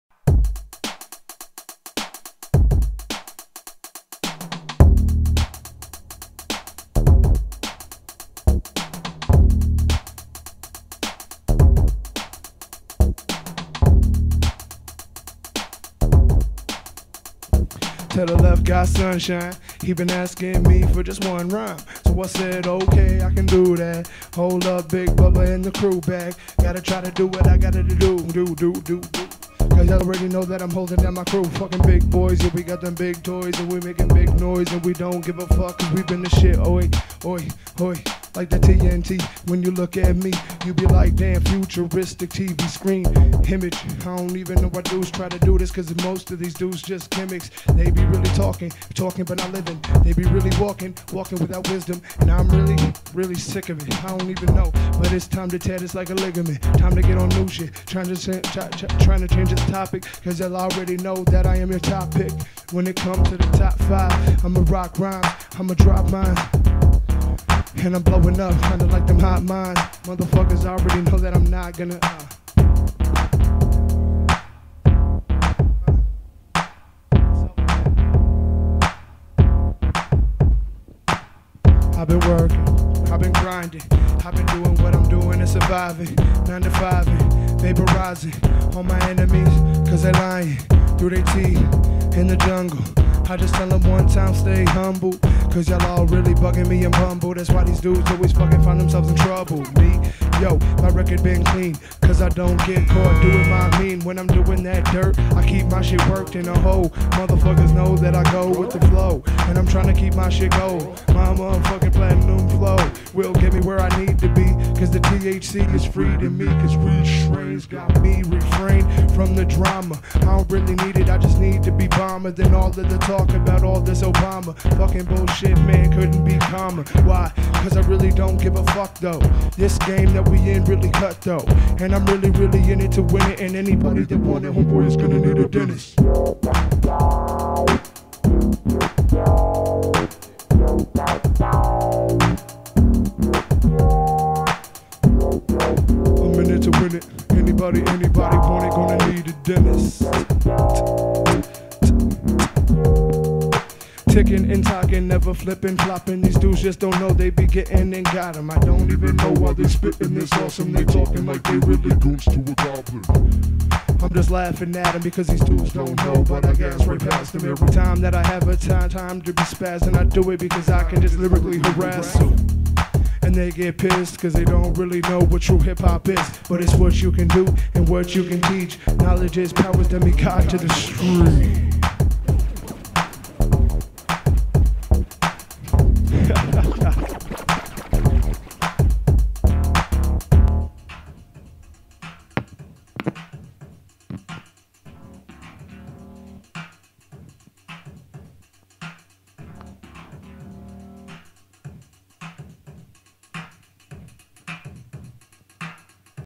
BIG BUBBA - Hardware Hip-Hop 001
The first video of a new series called "Hardware Hip-Hop" which looks at musical improvisation from a rap/hip-hop viewpoint utilizing electronic instruments and effects.

Featured in this video is Big Bubba, a prodigy in the making. Keep an eye out for more freestyles with this articulate kat!

Instruments:
KORG X-50 + AKAI XR-20 - BOSS RC-50 - Electro Harmonix - Micro Q-Tron - MXR Dyna Comp.
Big Bubba's Larynx - Shure SM57 - BOSS PS-5 - Line 6 DL4.
All signal being processed by a Scarlett 18i20 Digital-Audio Converter/USB Interface

TheSunshineGroup.bandcamp.com